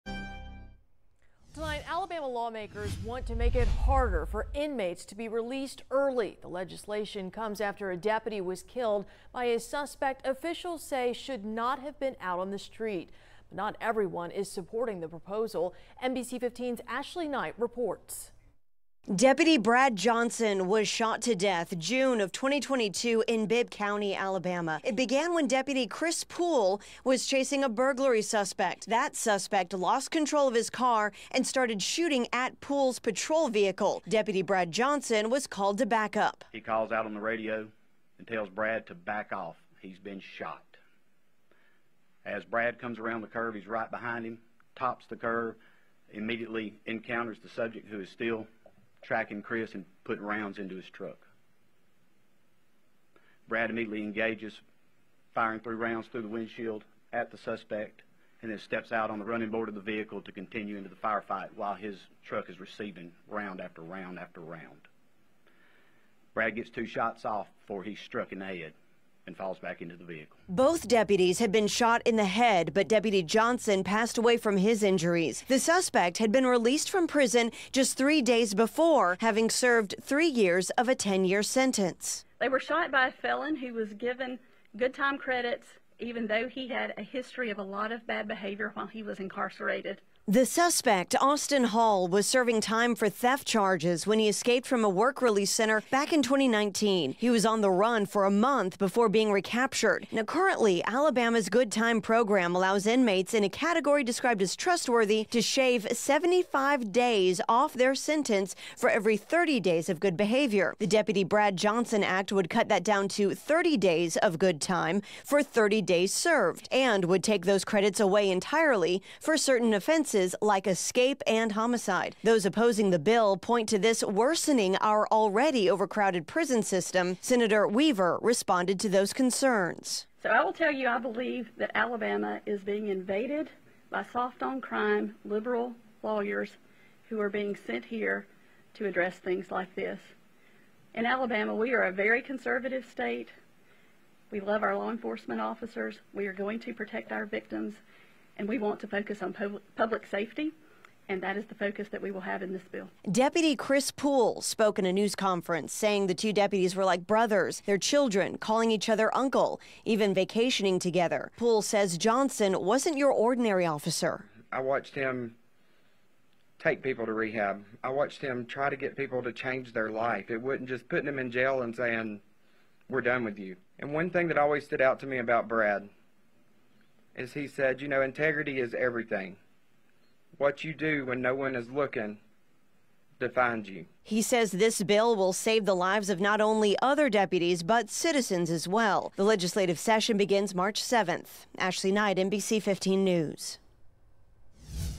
Proposed Alabama legislation would reform 'good time' law - NBC 15 WPMI
Proposed state legislation would make it more difficult for certain inmates to earn early release. The legislation comes after a deputy was killed by a suspect officials say should not have been on the street. But not everyone is supporting the proposal.